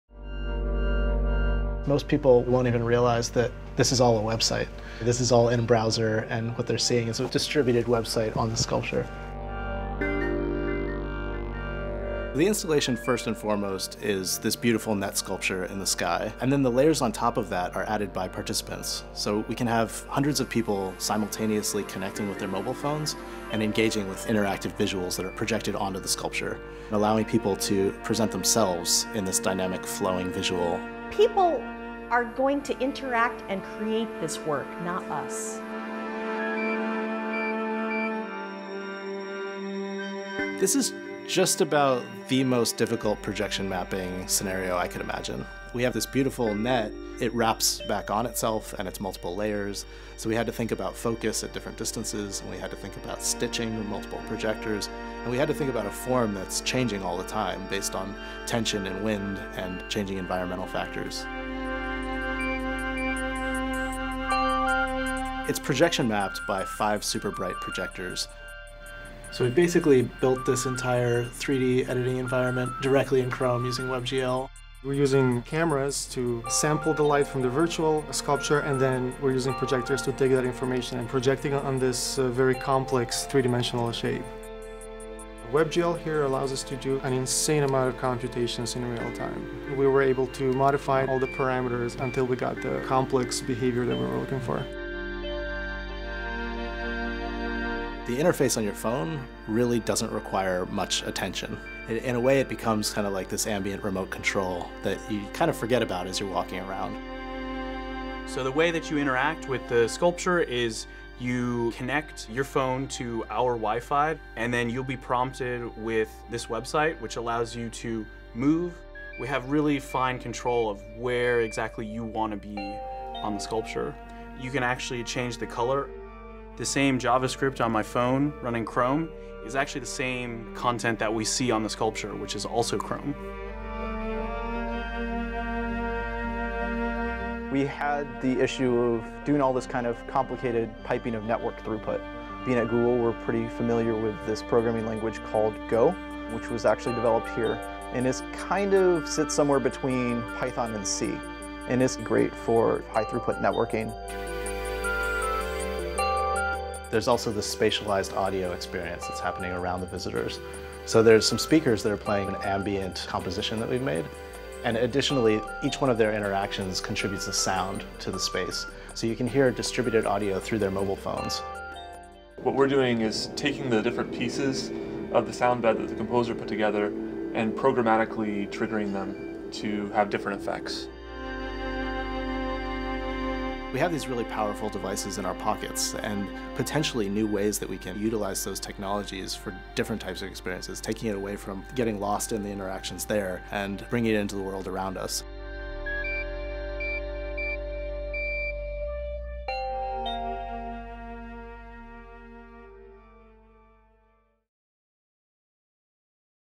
The Technology Behind Unnumbered Sparks
Learn about the technology behind Unnumbered Sparks, a monumental interactive sculpture in Vancouver, Canada created by artists Janet Echelman and Aaron Koblin for TED's 30th anniversary. Choreographed by visitors in real time through their mobile devices, at night, the sculpture becomes a crowd-controlled visual artwork on a giant, floating canvas powered by Google.